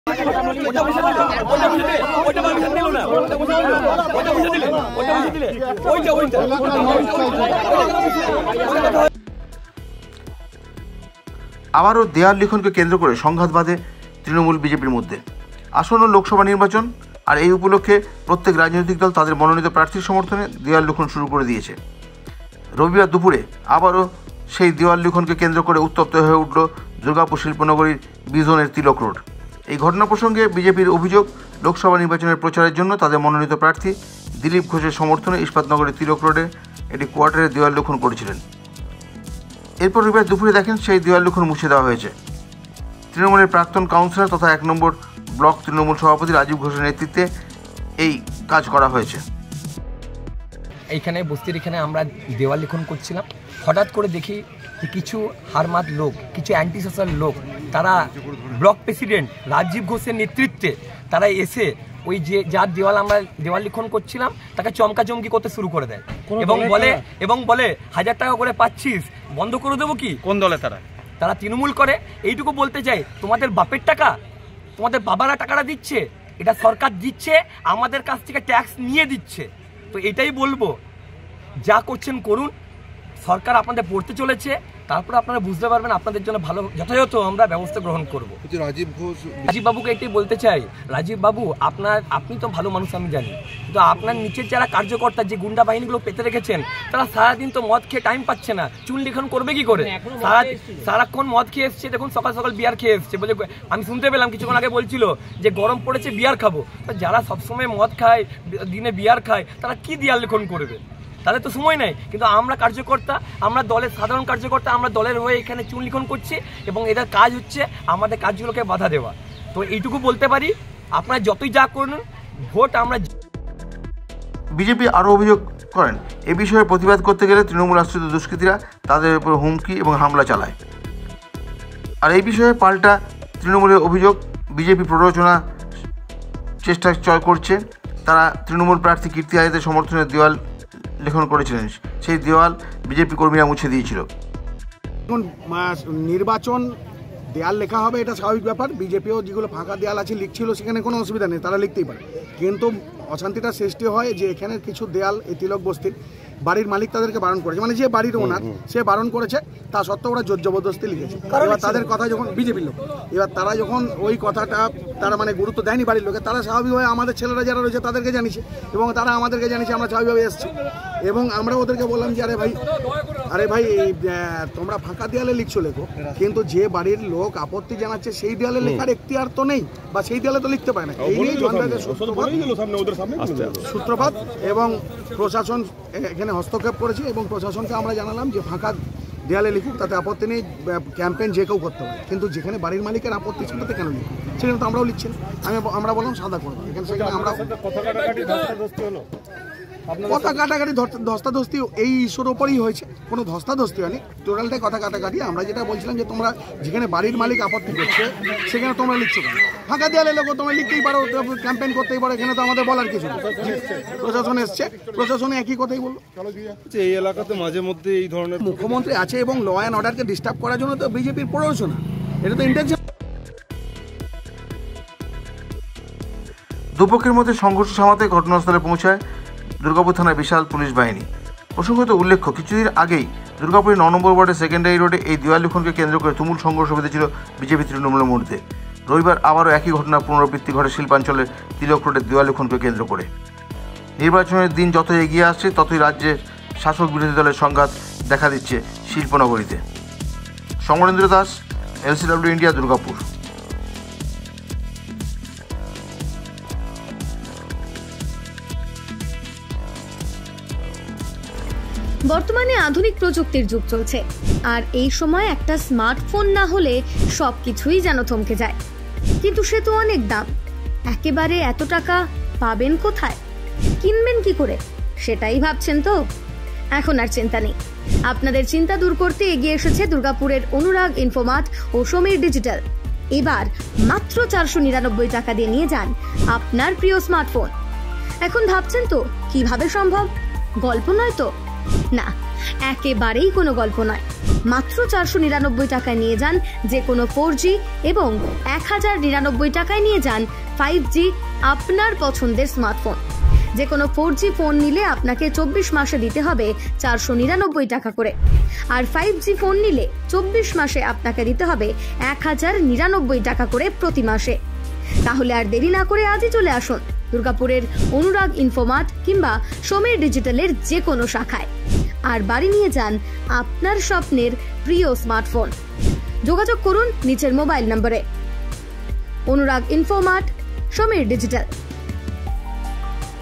দেওয়াল লিখন কে কেন্দ্র করে চরম সংঘাত তৃণমূল বিজেপির মধ্যে
দু'দলের এই সংঘর্ষ থামাতে ঘটনাস্থলে পৌঁছায় দুর্গাপুর থানার বিশাল পুলিশ বাহিনী